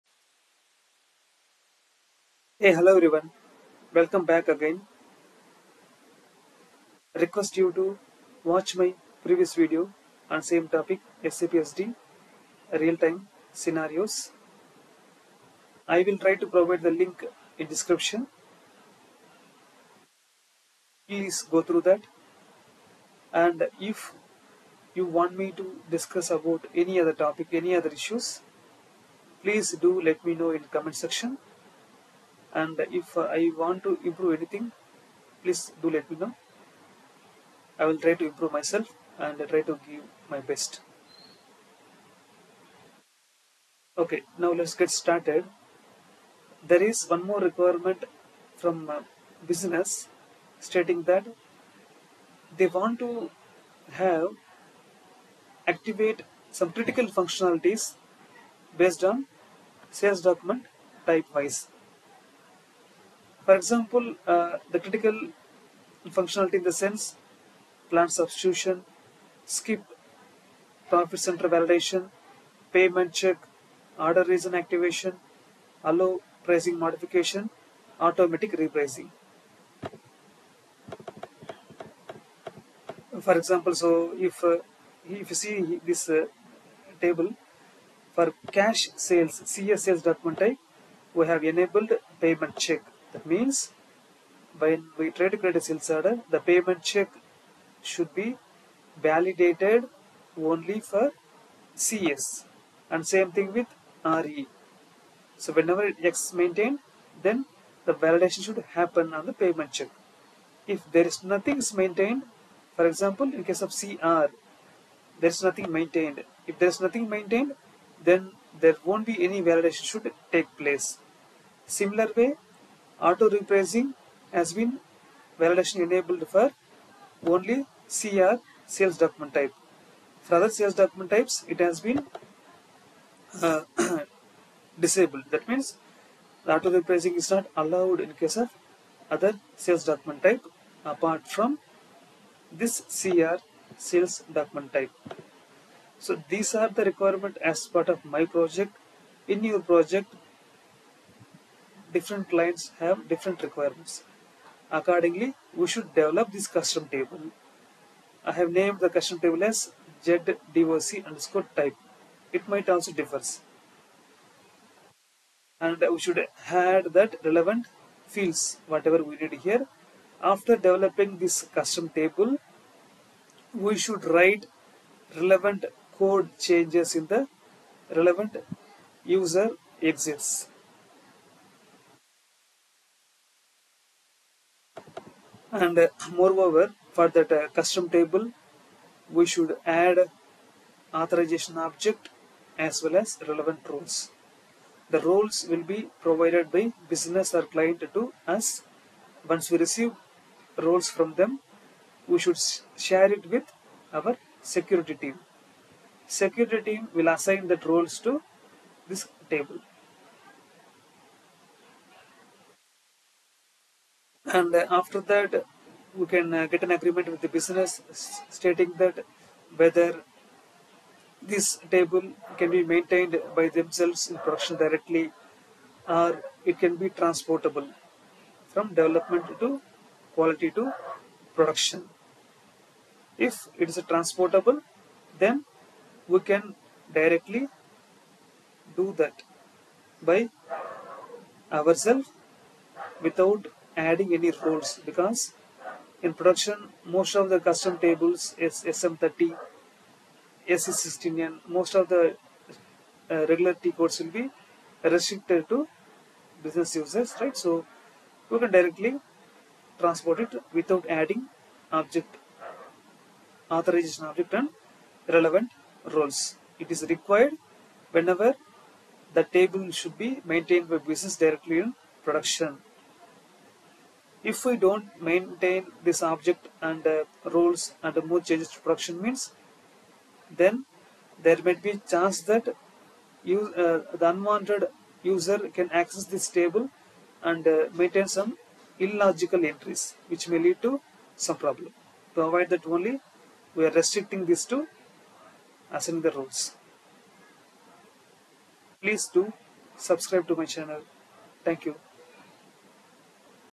SAP SD Real Time Scenarios (2023) - Sales Document type Wise Activation
In this session, SAP SD AND ABAP LEAGUE SUPPORT Raj is going to discuss about SAP SD REAL TIME SCENARIOS (SAP SD Business process scenarios). In other words we can say that SAP SD real time client requirements activation based on sales document type wise. Its good to know few SAP SD REALTIME SCENARIOS before we take interview.

Discussing about SAP SD REAL TIME scenarios:
Requirement 2: Client requirement is to activate some critical functionality based on sales document type wise.
Like in case of below situations it might require:
• Plant substitution
• Skip profit center validation
• Payment check
• Order reason activation
• Allow pricing modification!
• Automatic repricing
Solution: Need to develop custom table (ZDOC_TYPE) with required fields and required changes should be done in relevant user exits.